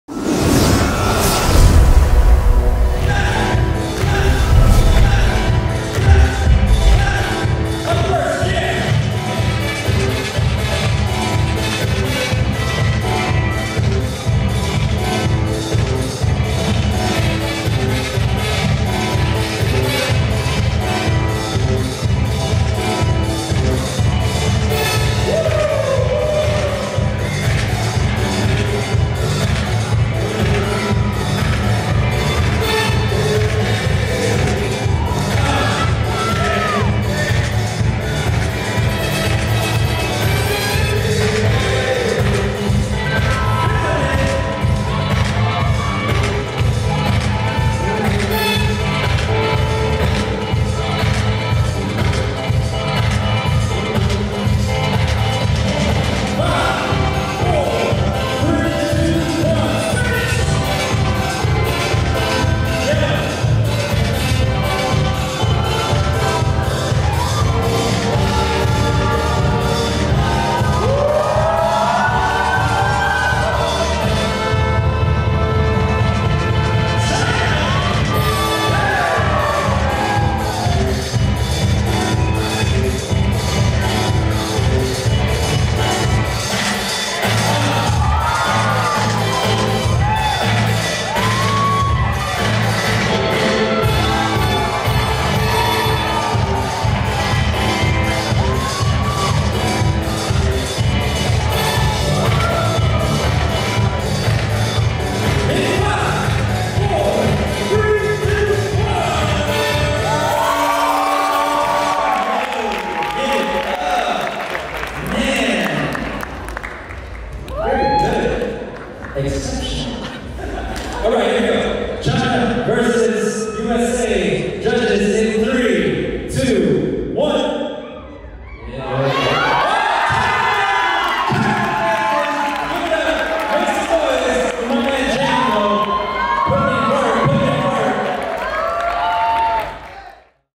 Cary - China vs Jam - USA | HHI's 2018 World Locking Battles Top 8
Cary (213) - China vs Jam (222) - USA
HHI2018 World Locking Battles. (Top 8)
Hip Hop International's 2018 World Battles!
(1vs1 Locking, Popping & Breaking and 2vs2 All Styles Battles)  Battles were held at the Arizona Grand Resort & Spa in Phoenix, Arizona.